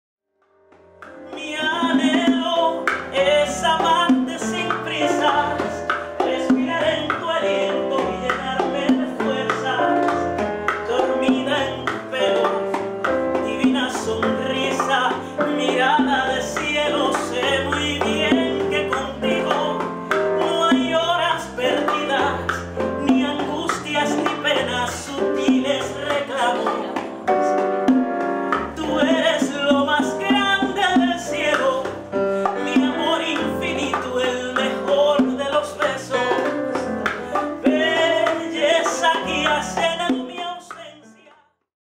Cicle 'Música en Vena' a l'Hospital Germans Trias
Concert de Cuanga Trío a la Unitat d'Hemodiàlisi de l'Hospital Germans Trias, en el marc de la col·laboració amb l'associació sense ànim de lucre Música en Vena.